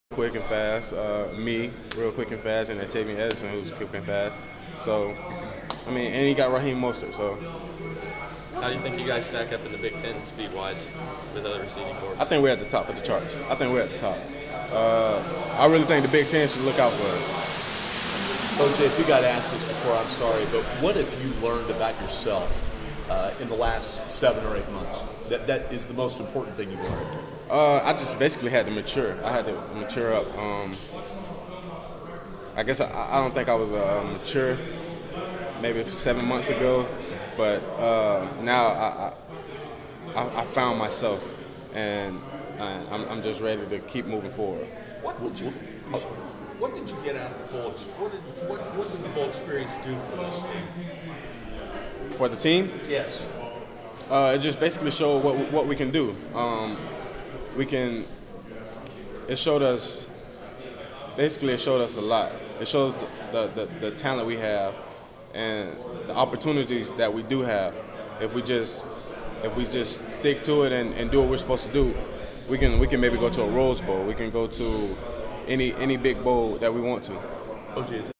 Purdue's Ross embraces responsibilities
Purdue junior receiver O.J. Ross says he's regained his football love and focus. Funny how a suspension and becoming a father can mature you.